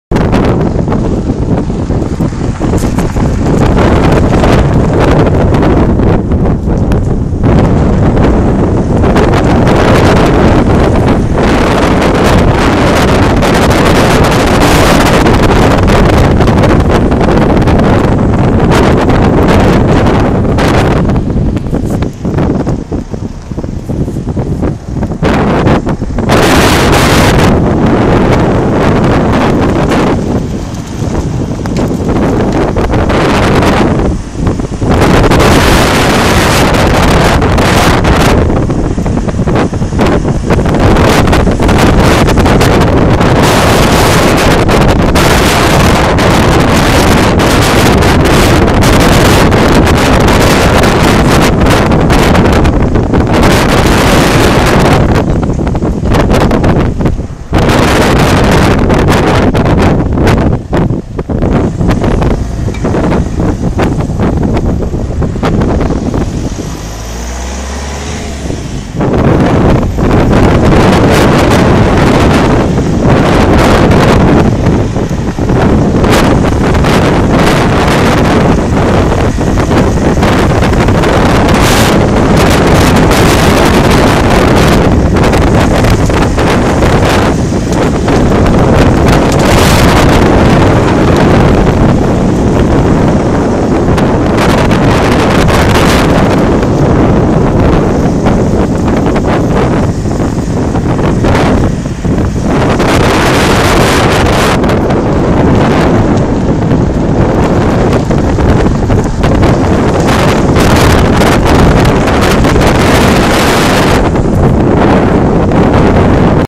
Village destroyed in war - Croatia
This ghost village is destroyed in war. It is in Croatia near Benkovac.